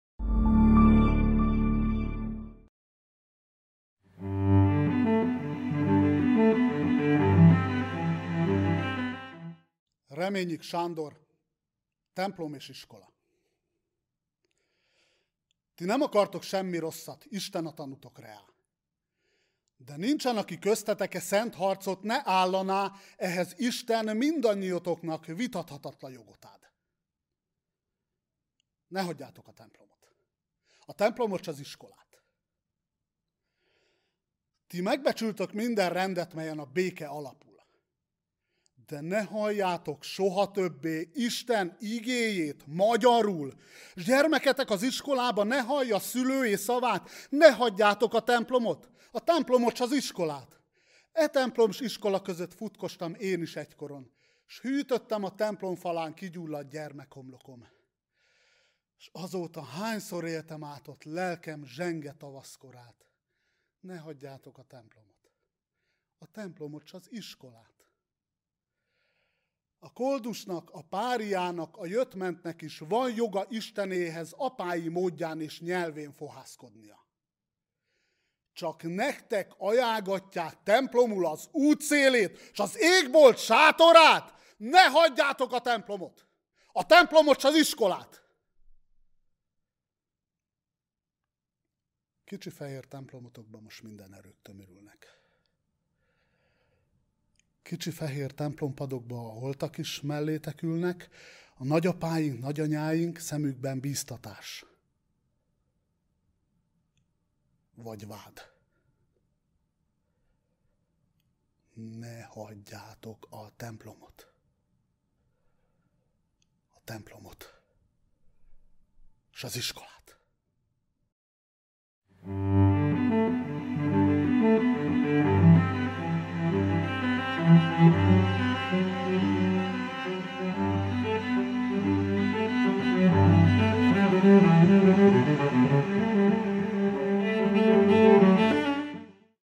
Kedvenc versem: Szabó György | Reményik Sándor: Templom és iskola
Online verses programsorozatunk következő részében Szabó György érzelmes szavalását hallhatjuk.

Várjuk azokat a versszerető és versmondó barátainkat, akik szívesen elmondják kedvenc versüket és vállalják, hogy a videofelvételt megosszuk.

2021